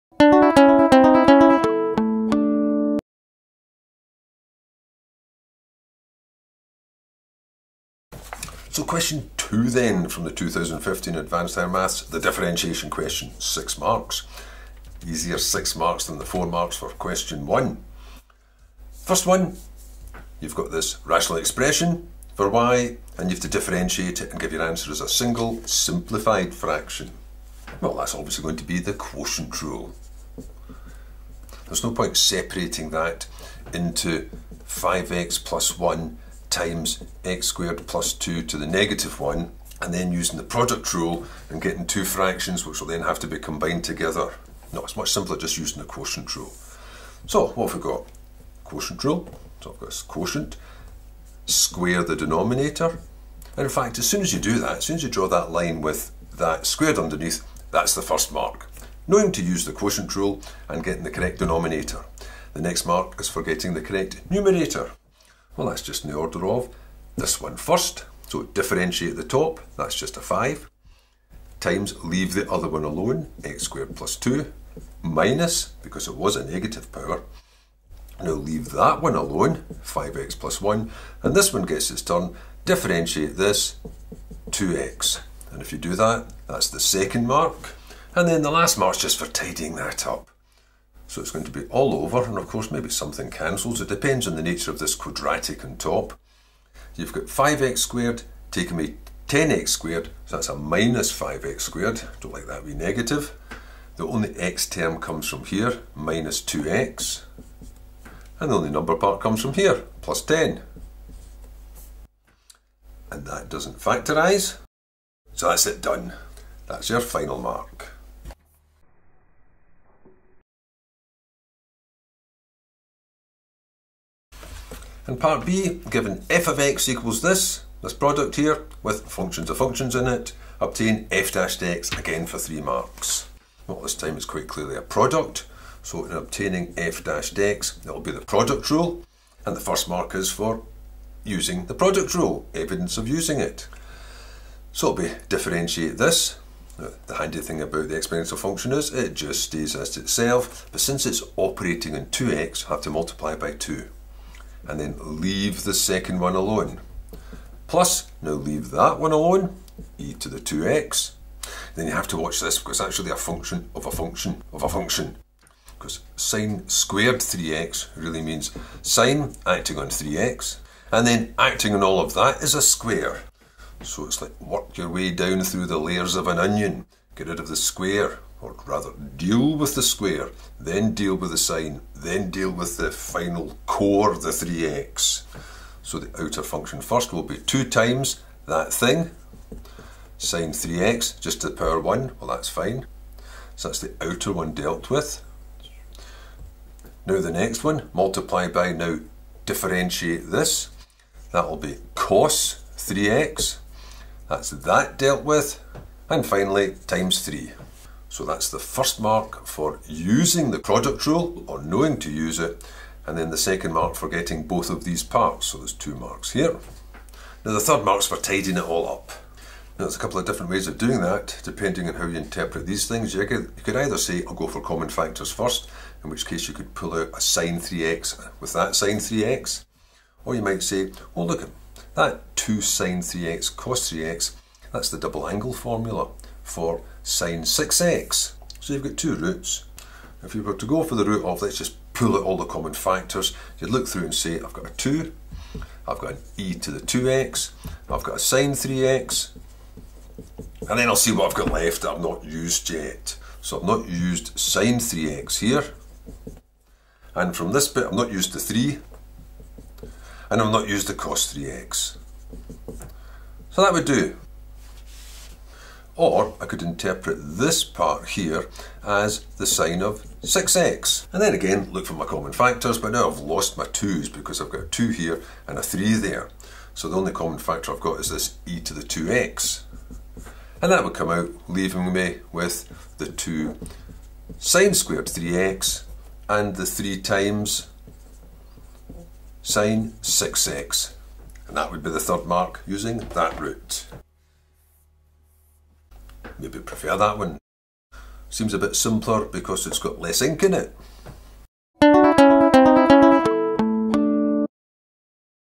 2015 SQA Advanced Higher Mathematics No. 2 Differentiation
Differentiate two given functions.

Instructional exercise consisting of question 2 from the 2015 SQA Advanced Higher Mathematics examination.